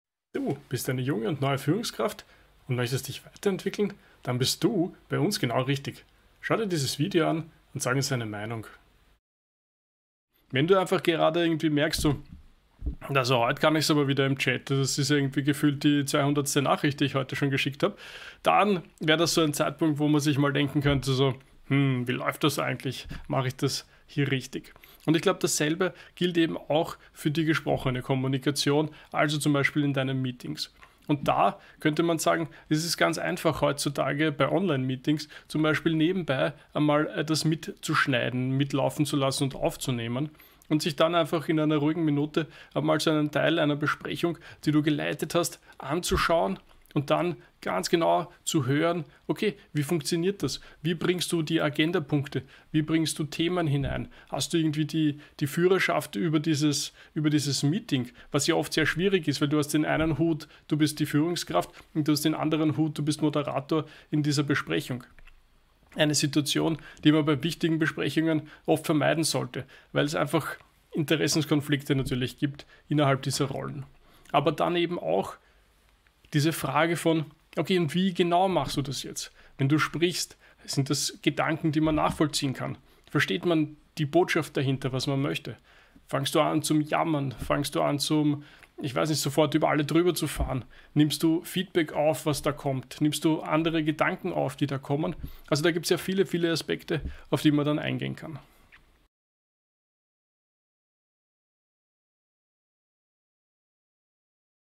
Evaluiere ich als Führungskraft meine Kommunikation?
In diesem kurzen Clip spricht Flo über die Notwendigkeit als Führungskraft die eigene gesprochene Kommunikation, insbesondere im Team-Besprechungen, zu evaluieren.